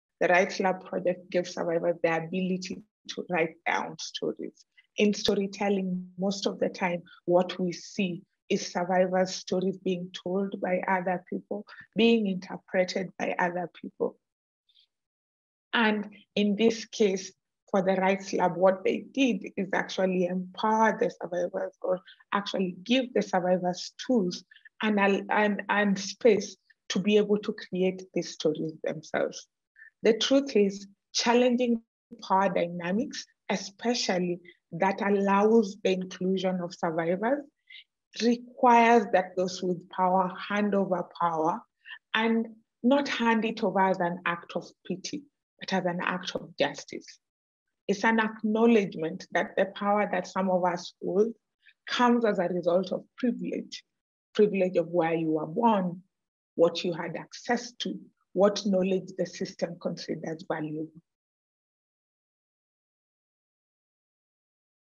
Sophie Otiende reflects on the Survivors Voices Stories and Images project
Sophie Otiende, Guest Curator with the Antislavery Knowledge Network, reflects on the Survivors Voices, Stories and Images project led by Azadi Kenya with the Rightslab, University of Nottingham.